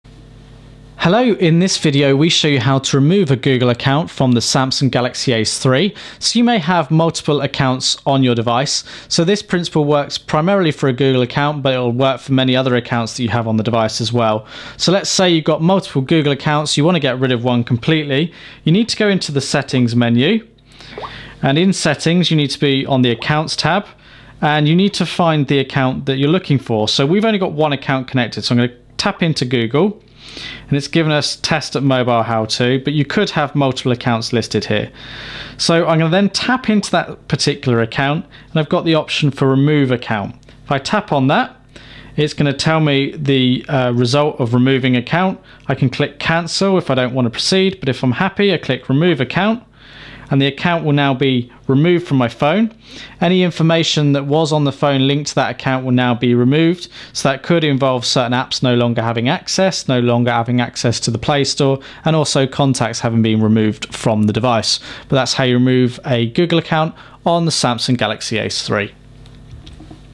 How To Remove A Google Account - Samsung Galaxy Ace 3
A video how to, tutorial, guide on removing a Google Account on the Samsung Galaxy Ace 3.